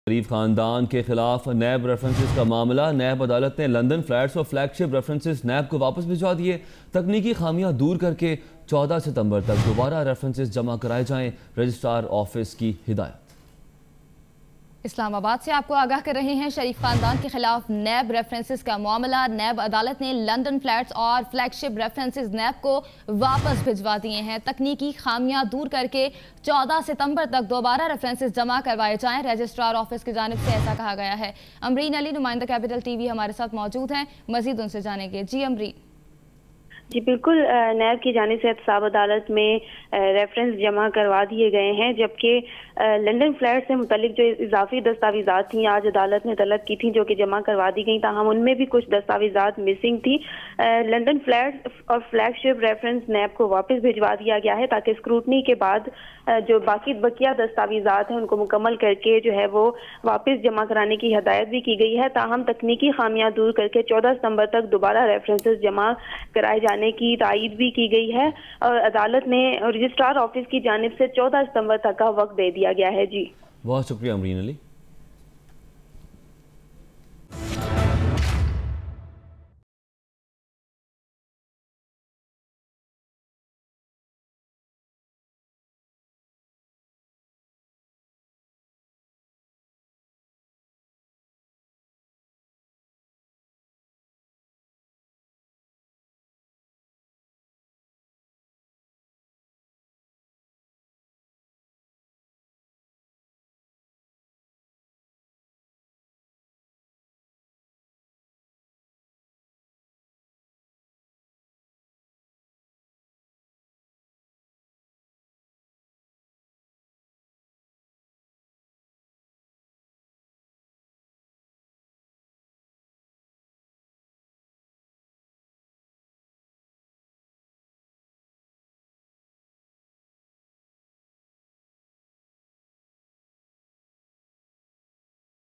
Court directs NAB to submit corrected references again
The National Accountability Bureau submitted on Monday additional documents to the accountability court’s registrar office, where the process of scrutiny of the references against the Sharif family continues. Two of the references had errors in them. Many pages of the references do not have page numbers mentioned and some of them have the wrong number printed. For more details, watch the video